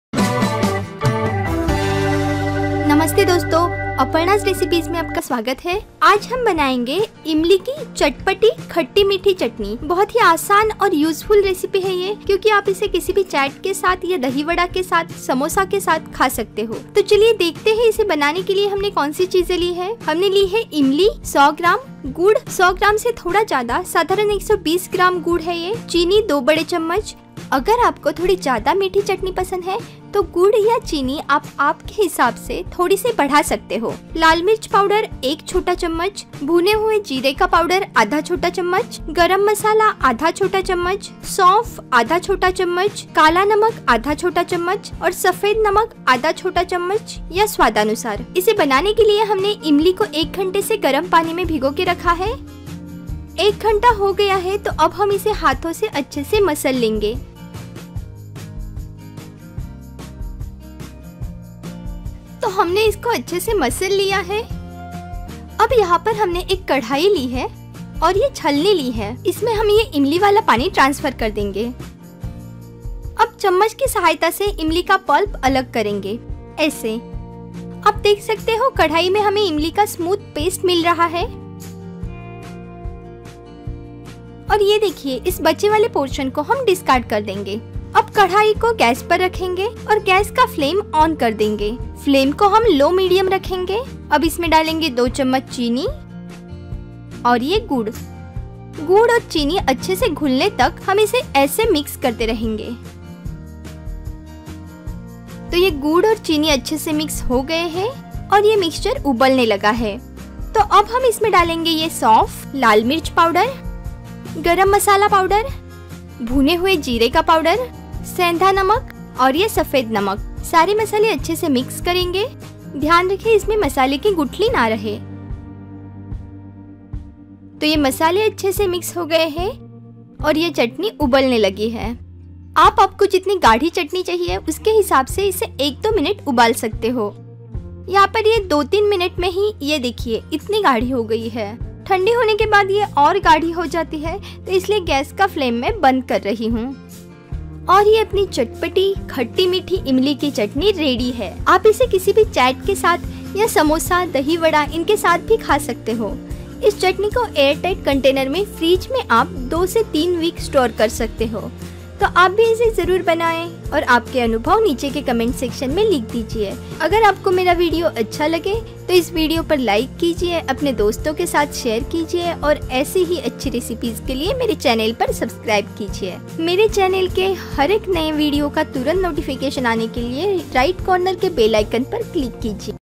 Sweet and sour Tamarind Chutney | Imali ki Khatti Mithi Chutney
Easy and tasty sweet and sour tamarind chutney for chat, dahi wada, samosa etc. You can store it for 2-3 weeks in air tight container in refrigerator.